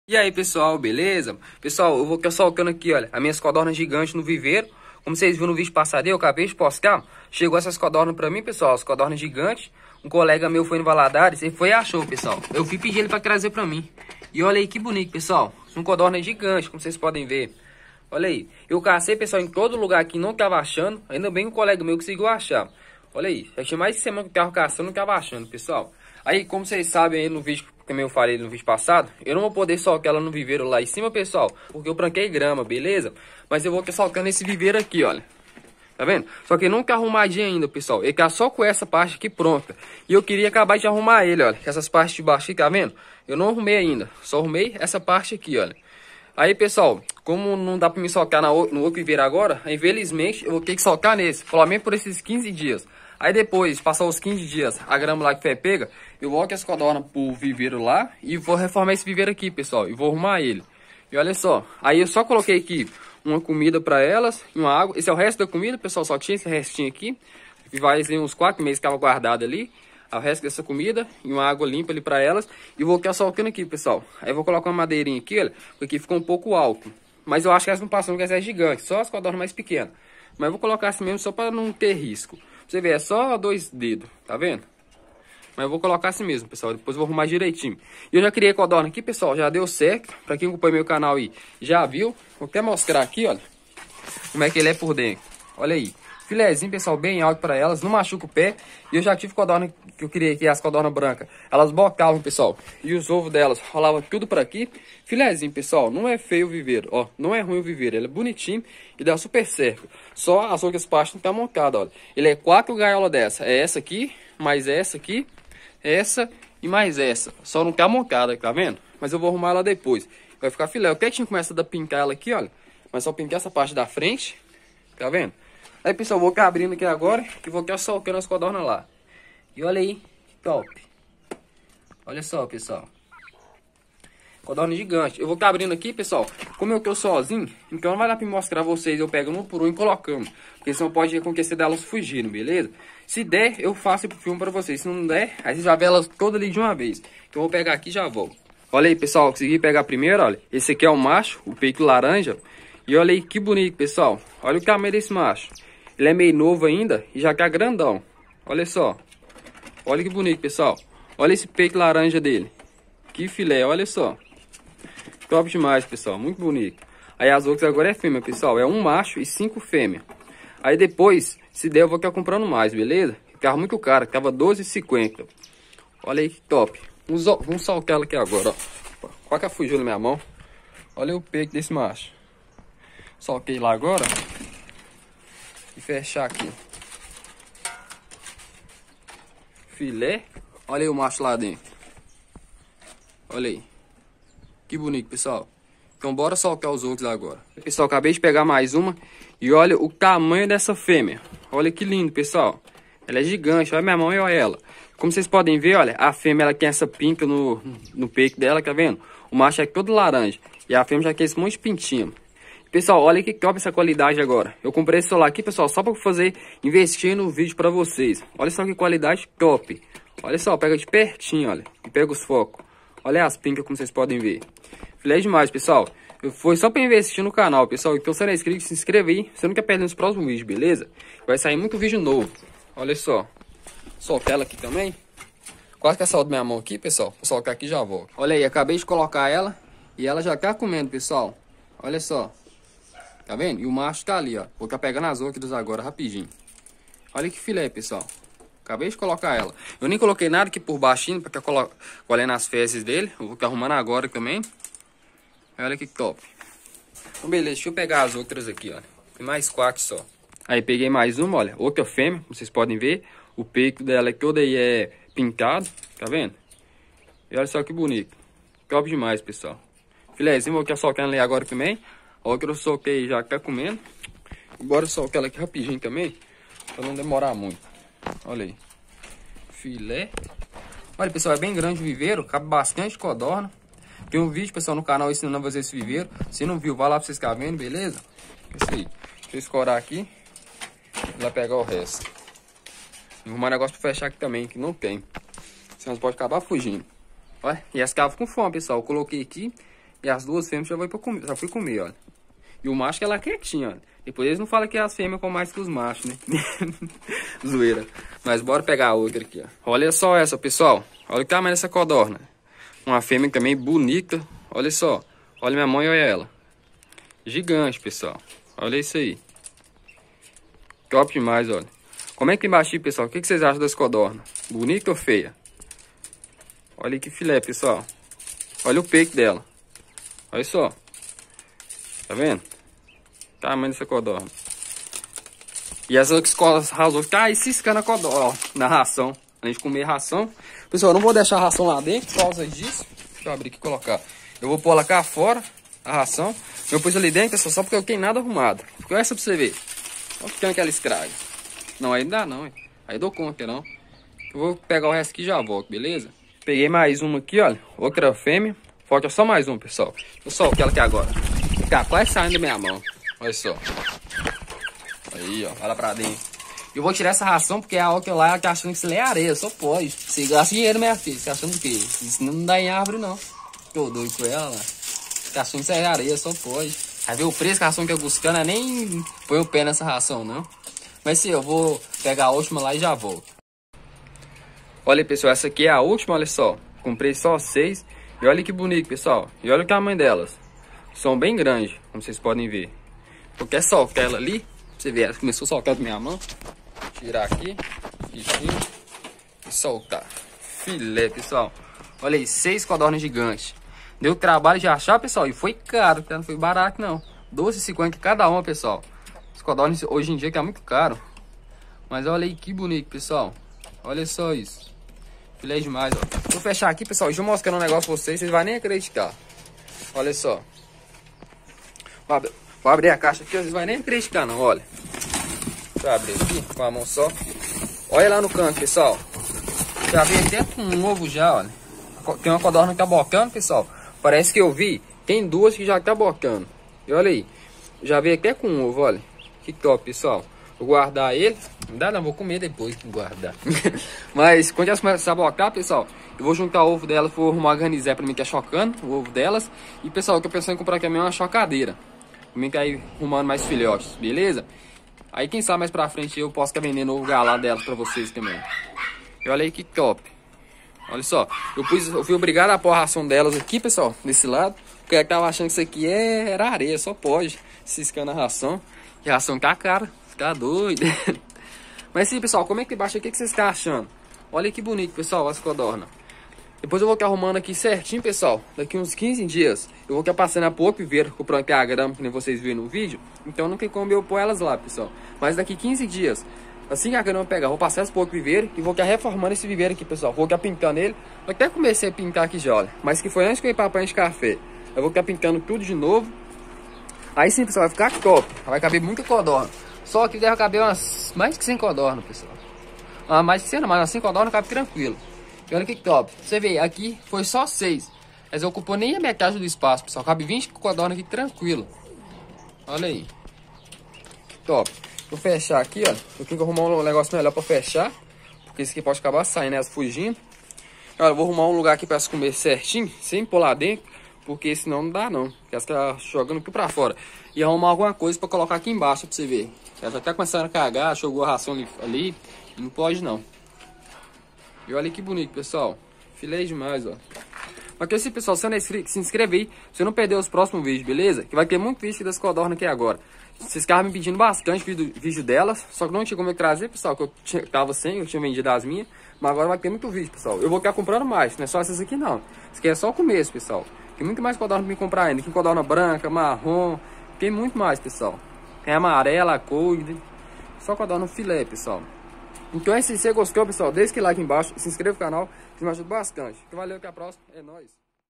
Sotando codorna gigante no viveiro
Nesse videio eu vou ta soltando minha codornas novas no meu viveiro.